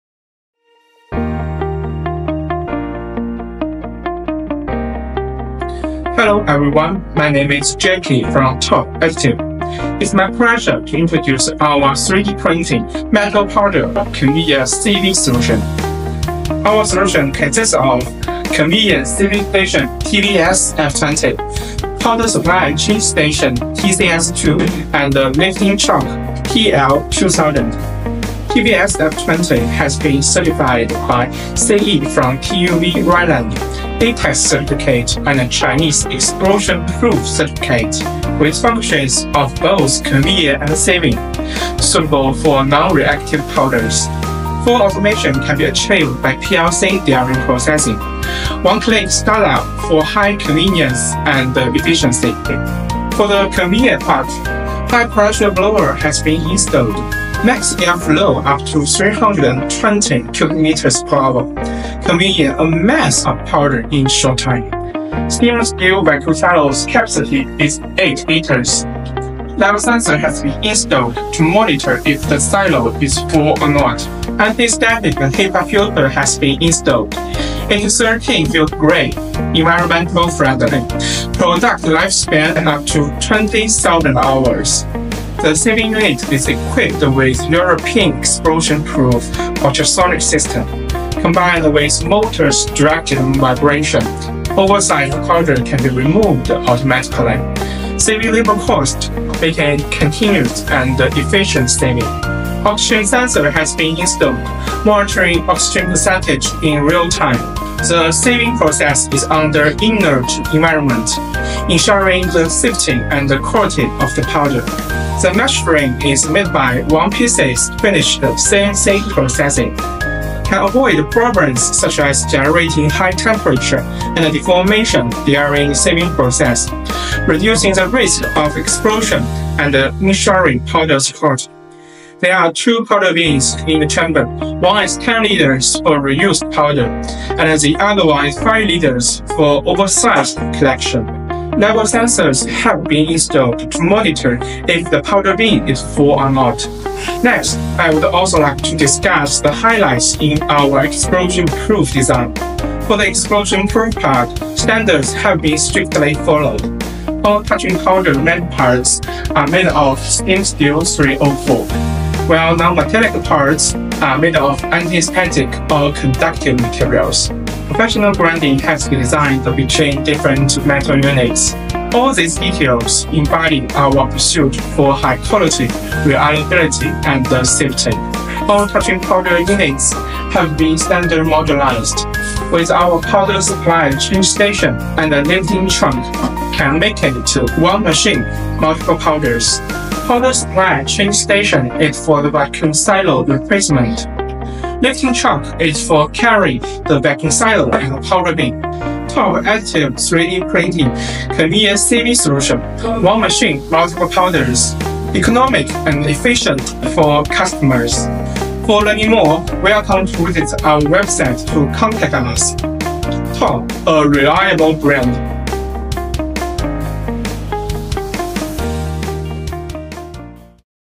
TOP Additive 3D printing metal powder conveyor sieving solution
Consist of:
*Conveyor sieving station TVS-F20 E VAD
*Powder supply change station TCS-2
*Lifting truck TL 2000
CE certificate, ATEX certificate
One machine for multiple powders
Economic and efficient